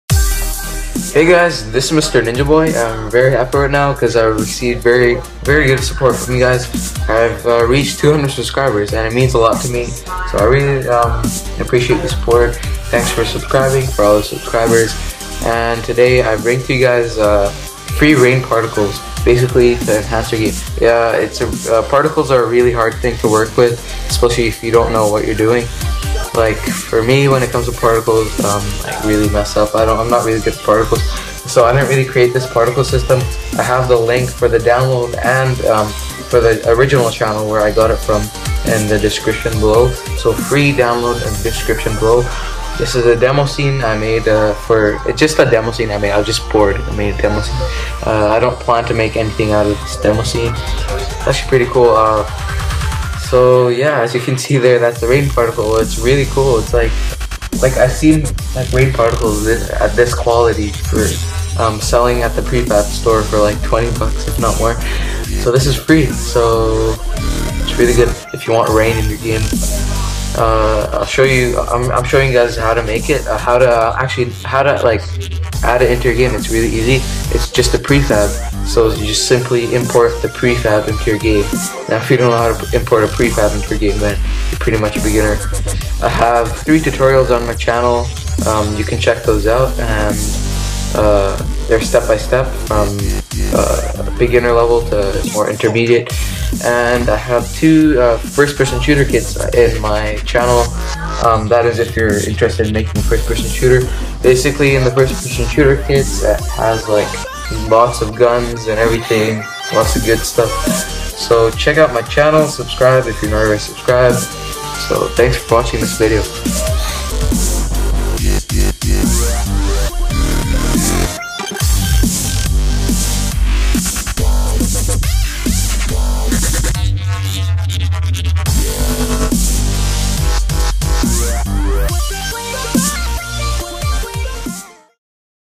UNITY3D Rain Particle System FREE!
Unity Rain Particle System ready to use in your game. Particles in unity are an excellent way to add some nice detail to your game. And in this video, I show you how to add a rain emission effect from your cloudy skybox.